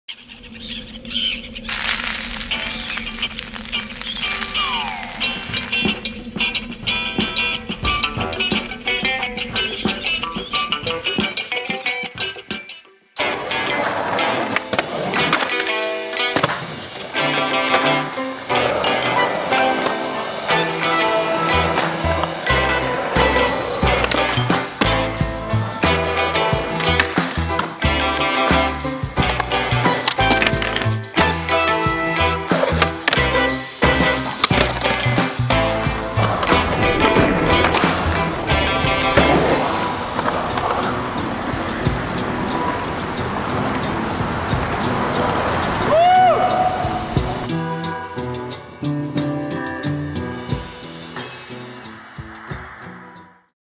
Jamie Palmore VX Moment
Here's a couple of VX 1000 clips we filmed recently and a few from the past of our man Jamie Palmore representing Osiris Shoes,Sk8mafia and JSLV Clothing. Peep It!